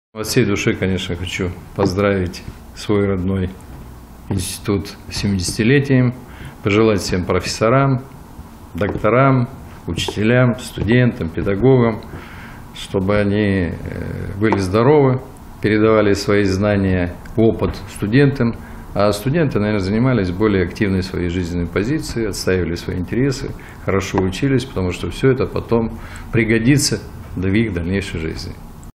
Юрий Карликанов
Юрий Карликанов - первый заместитель председателя ЗСО челябинской области. Выпускник ЧПИ(ЮУрГУ)